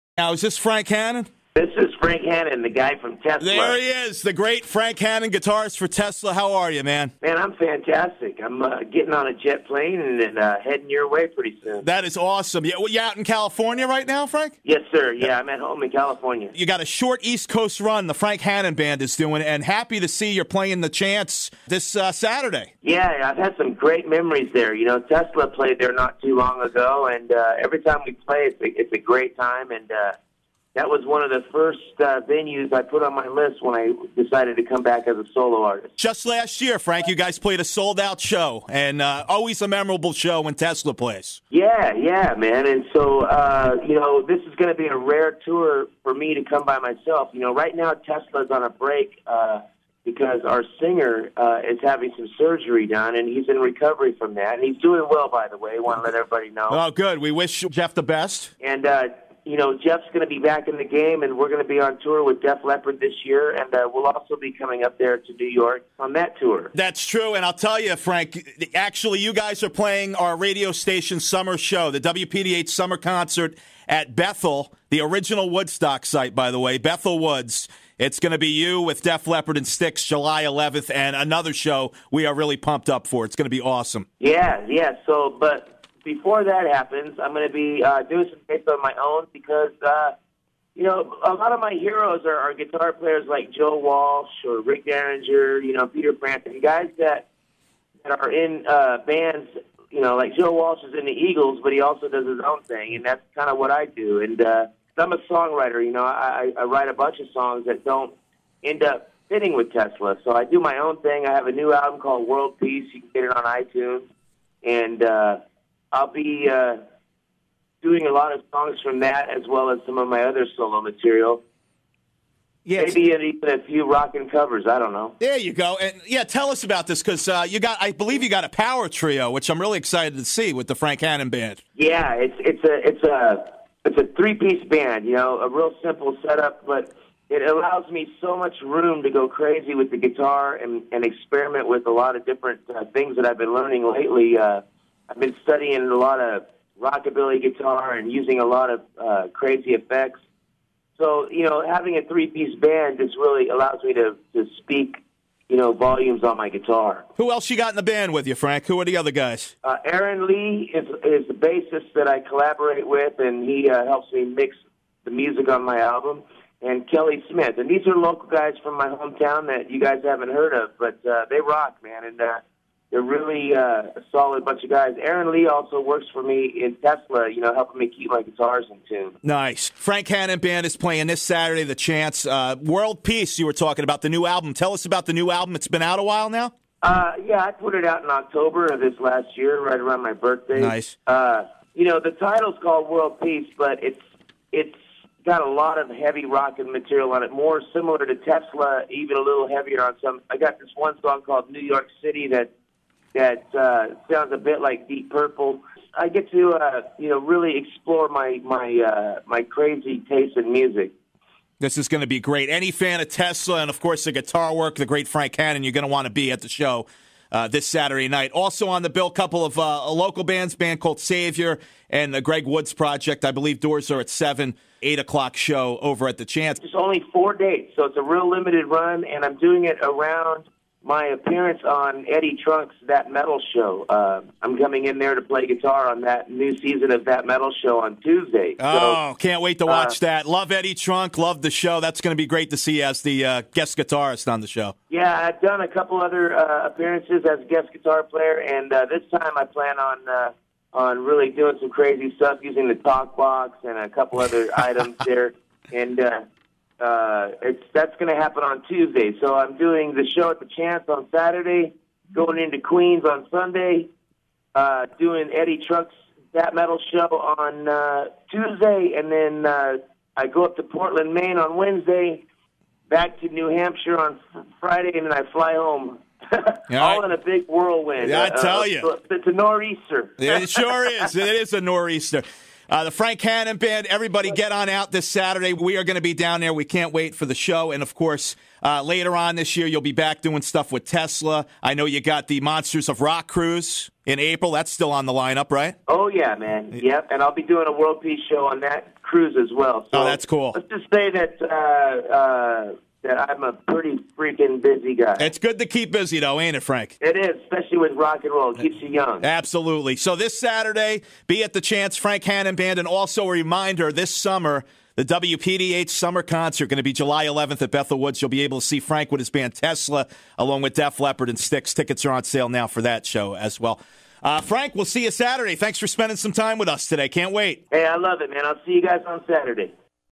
Tesla's Frank Hannon Interviewed on WPDH
Tigman spoke with Frank Hannon of Tesla.